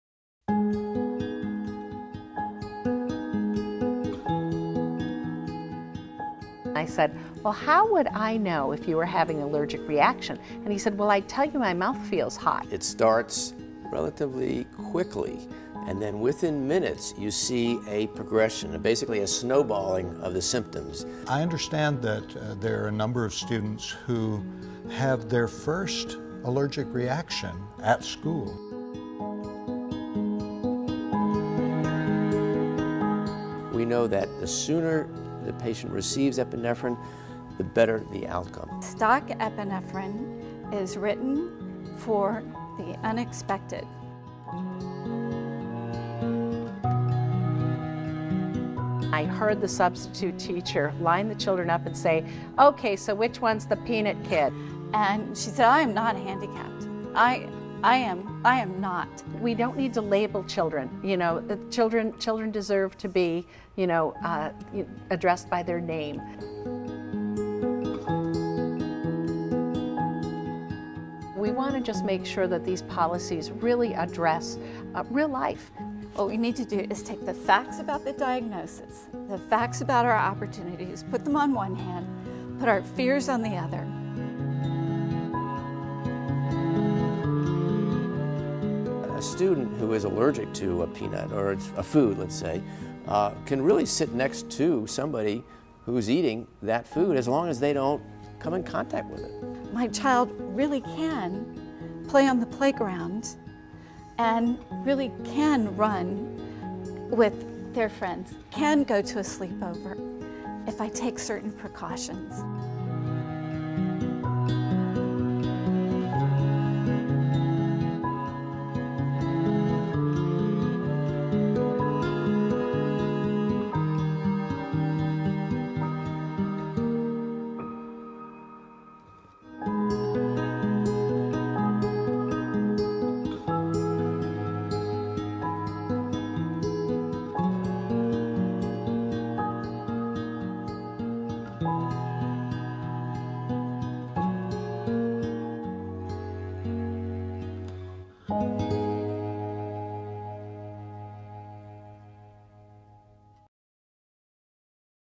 Epi Everywhere! Every Day!™ School-based Anaphylaxis Preparedness - Trailer
Epi Everywhere! Every Day!™ saves lives! When food allergy accidents happen, are students and educators prepared? Should state policies require schools to stock epinephrine auto-injectors? Does "peanut-free" mean safe at school?

Allergy & Asthma Network Mothers of Asthmatics, American College of Allergy Asthma & Immunology, American School Health Association and the National Association of School Nurses offer practical resources.